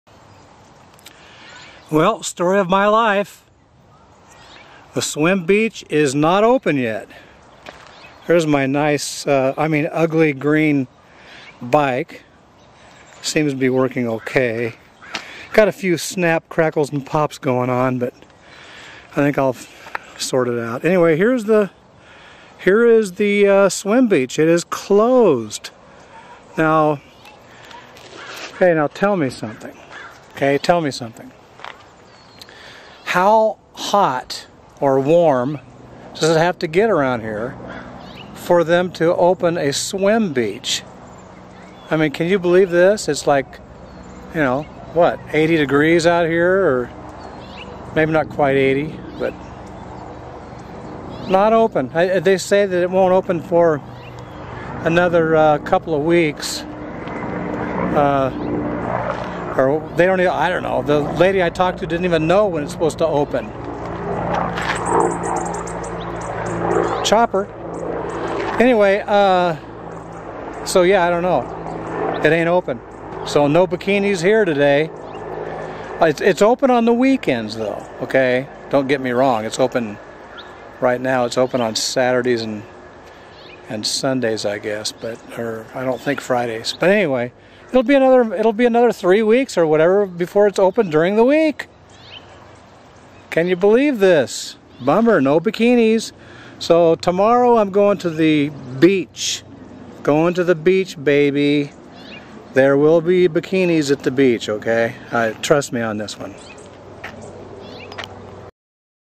No Bikinis Today: Swim Beach Closed
How hot does it have to get to open a swim beach around here? They should be open year-round here in Southern California!